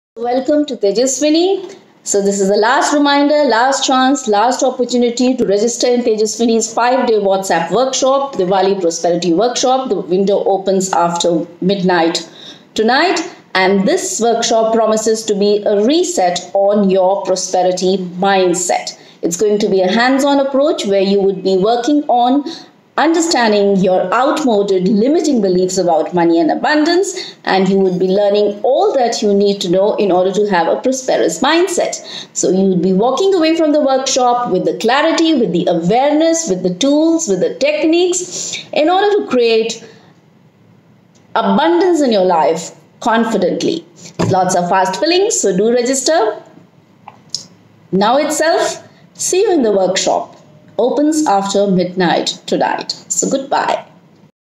Tejaswini's Diwali Prosperity WhatsApp Workshop Oct 21 - Oct 25
HURRY !! LAST CHANCE TO REGISTER
DIWALI PROSPERITY 5 DAY WHATSAPP WORKSHOP : OCT 21 - OCT 25.
Learn Techniques from Energy Healing & Neuro Linguistic Programming to Attract Energy of Money & Abundance in Your Life.
Workshop Window Opens After Midnight Tonight
To enroll👇🏾